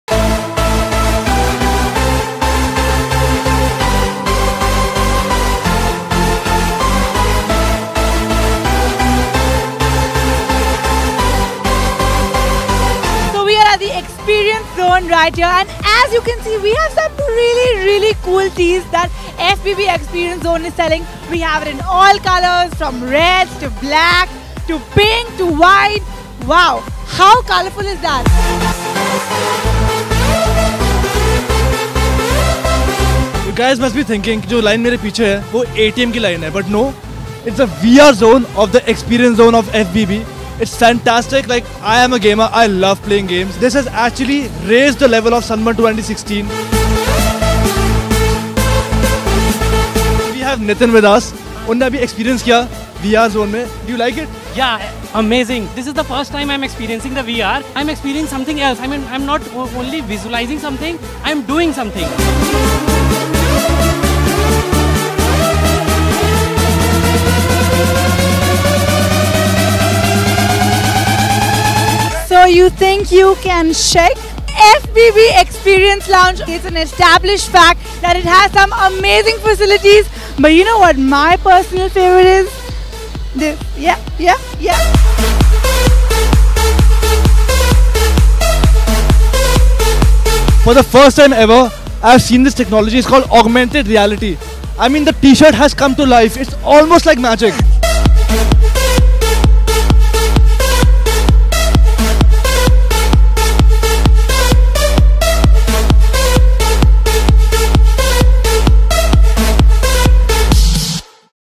Excitement at fbb Sunburn Expereince Zone
With virtual reality, augmented reality, trampolines, and our host Benfasha Soonawala and Varun Sood's favorite the dance booth, fbb made Sunburn 2016 fREAKINbLOODYbRILLIANT.